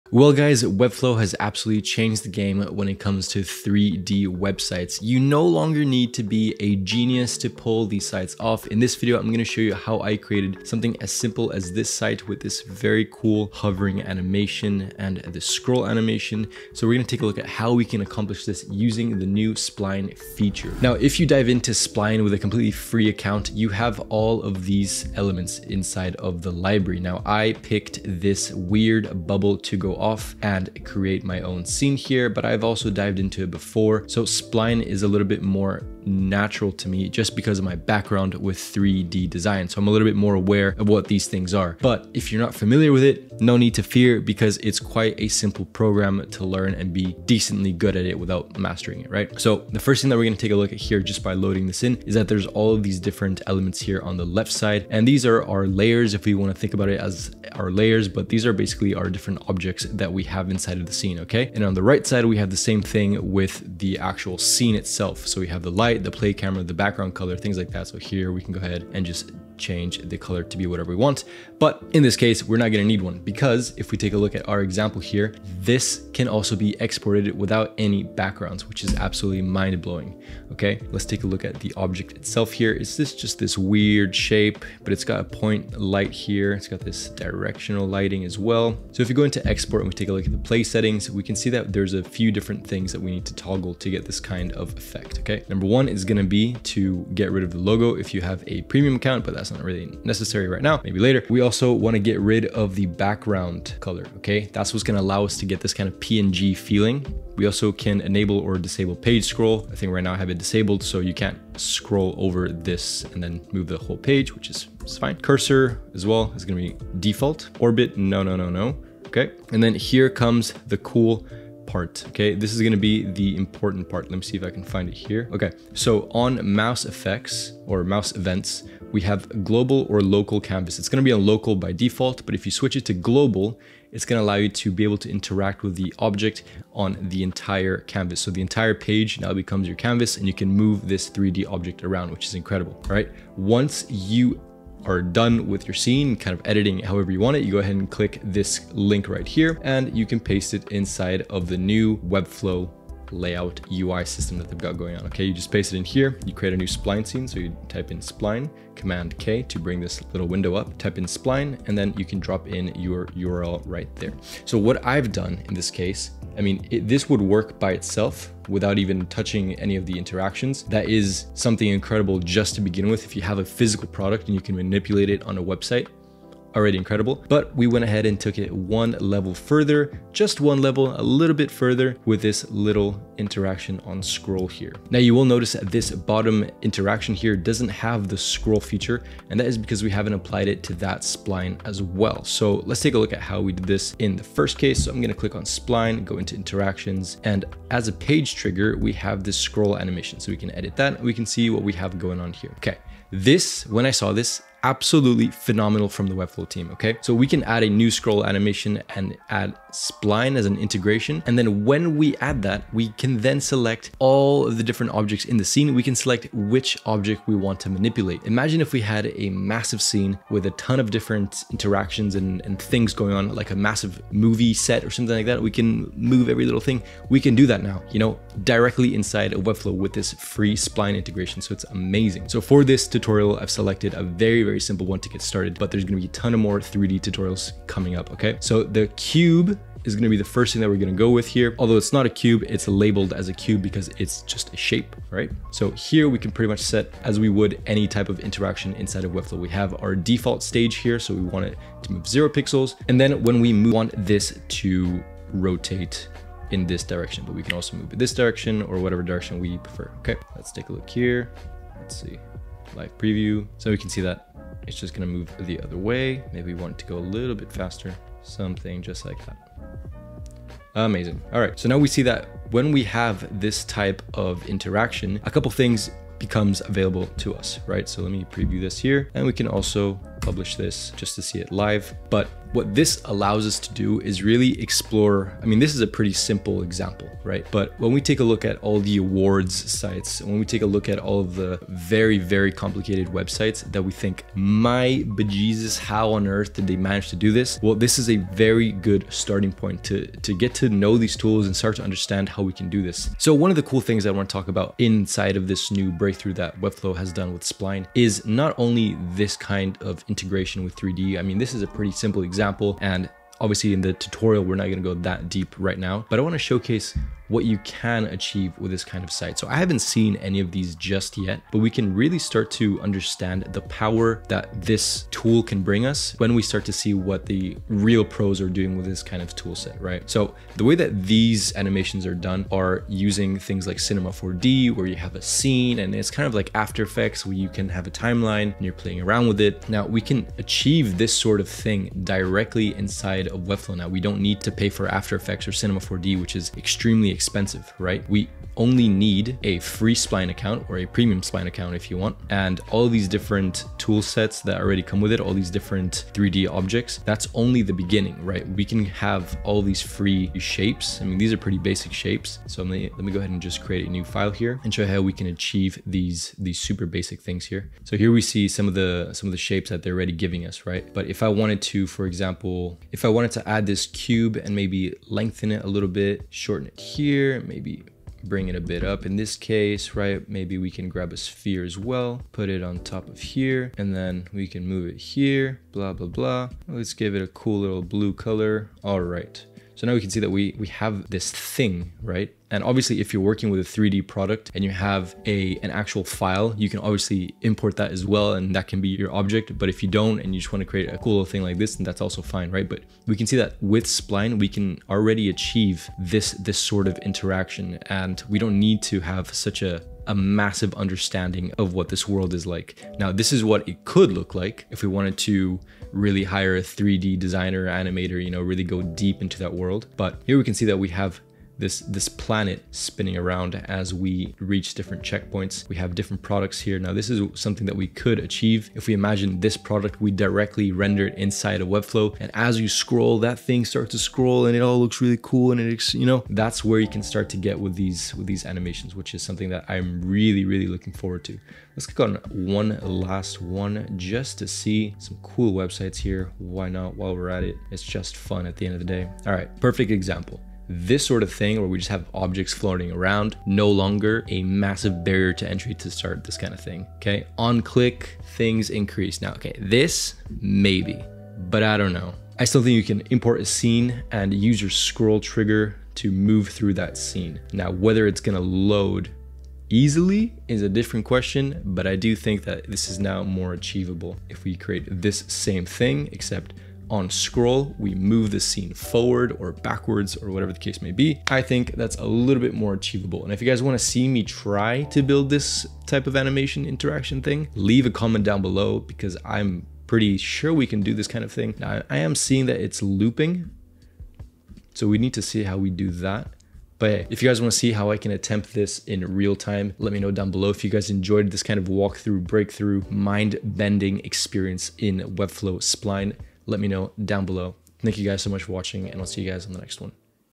How To Create 3D Website Designs With Webflow and Spline
In this video, I'm going to show you how to create 3D website designs with Webflow and Spline without typing a single line of code. Webflow has released a new update allowing users to integrate with Spline to be able to control animation features directly in program. This will allow designers to really up their game when it comes to 3d content and website designs.